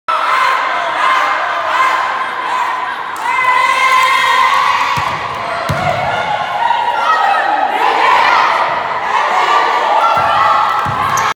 Lincoln vs. Sheepshead Bay High School
Congratulations to the Varsity Girls basketball team in defeating Sheepshead Bay HS in their Home Opener!